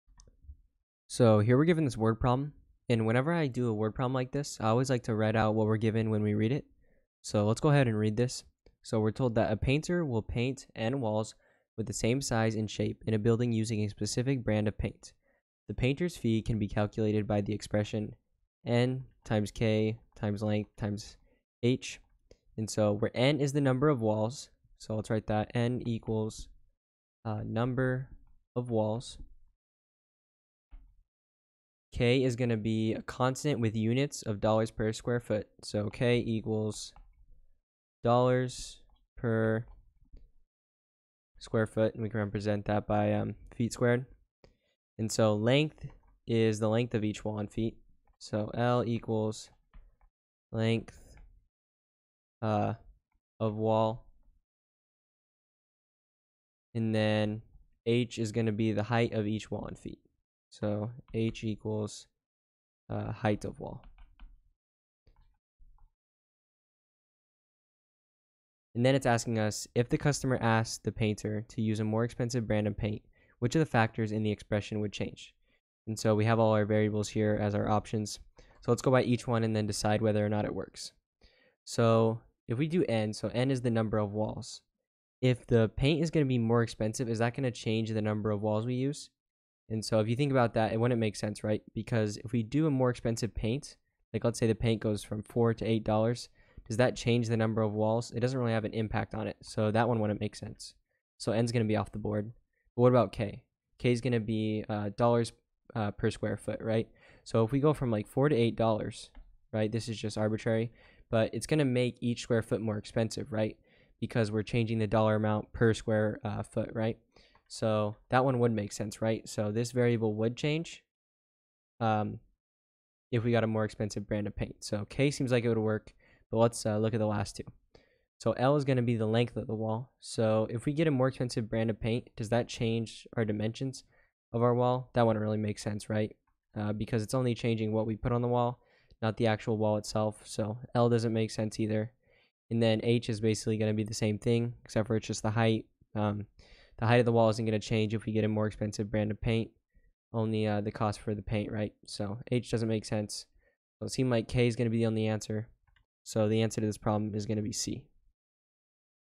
SAT Practice Test #3 Section 3: Math Test – No Calculator #1
A painter will paint n walls with the same size and
shape in a building using a specific brand of paint.
The painter’s fee can be calculated by the expression
nKAh, where n is the number of walls, K is a
constant with units of dollars per square foot, A is
the length of each wall in feet, and h is the height of
each wall in feet. If the customer asks the painter to
use a more expensive brand of paint, which of the
factors in the expression would change?
A) h
B) A
C) K
D) n .